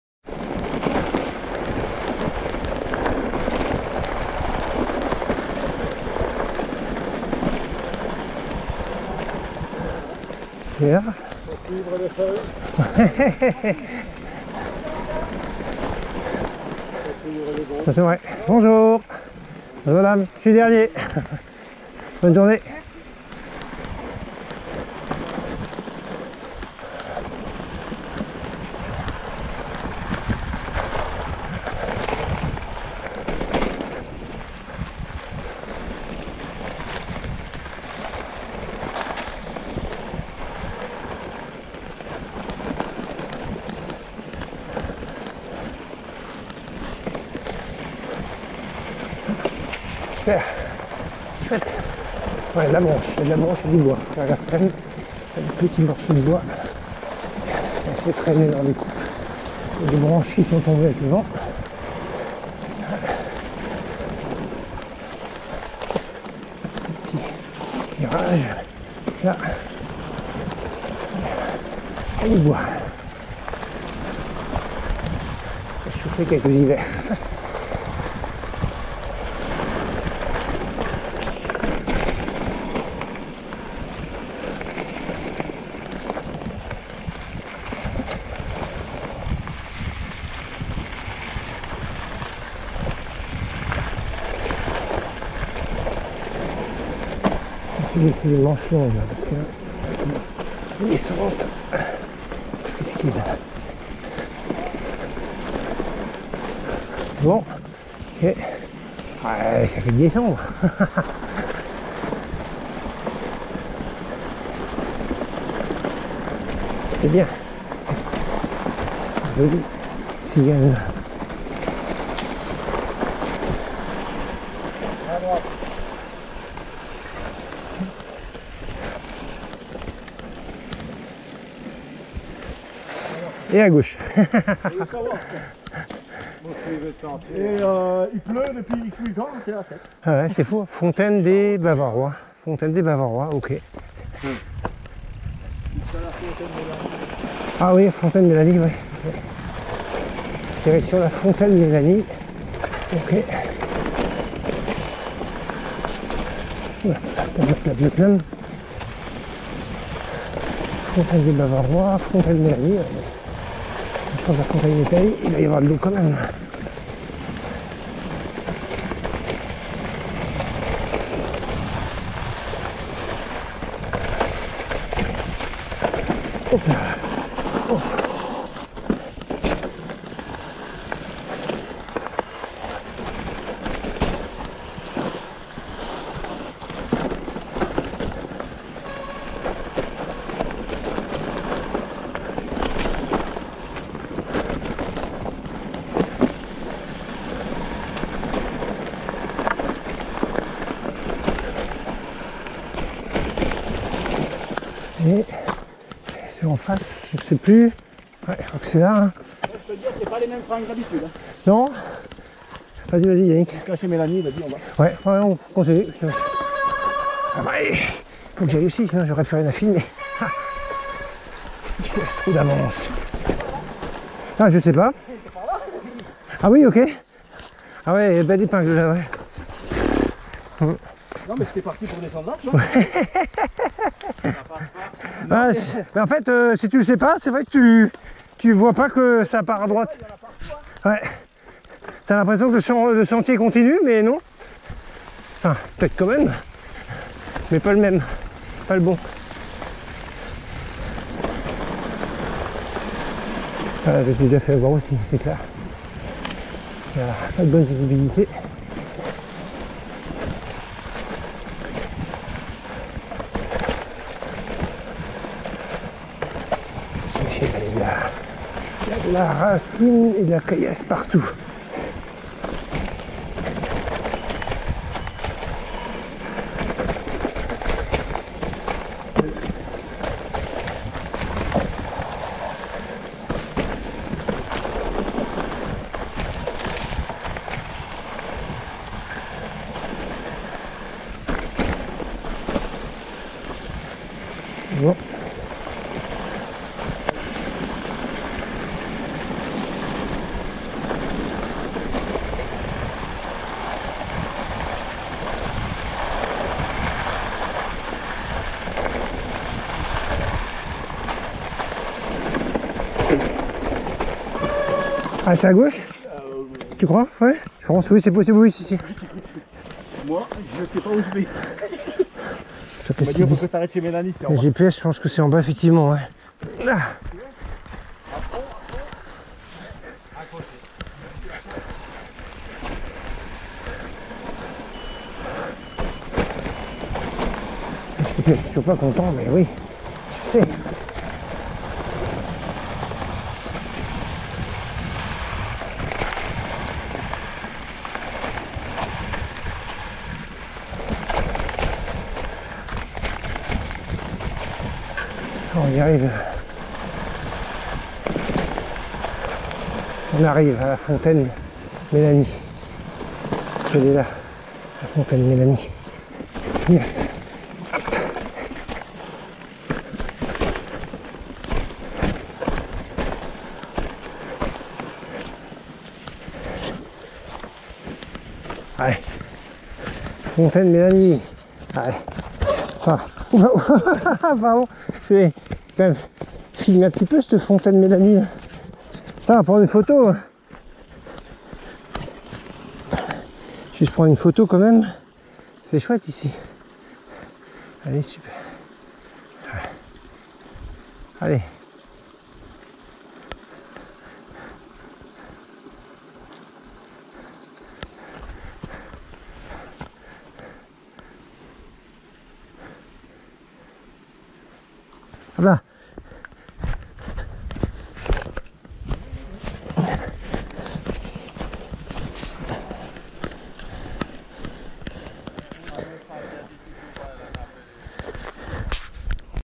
Ext 11, sortie VTT CC Wasselonne, départ Marmoutier, 11/11/2023
Descente sur single, fontaine des bavarois, fontaine Mélanie